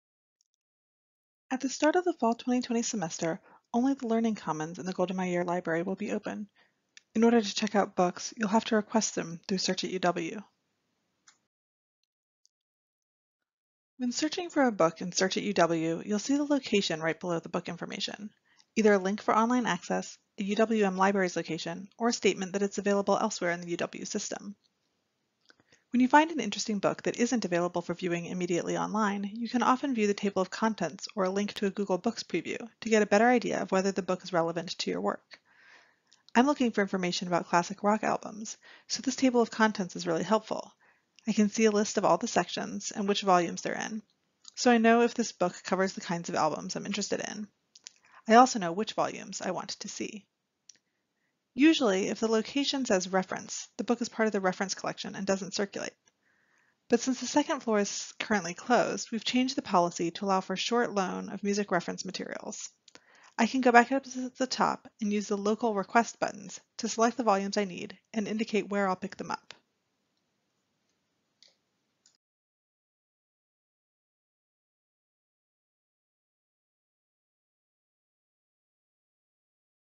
Tables of Contents and Local Requests
Using the Table of Contents to identify useful books and how to request books for pick-up during Fall 2020 semester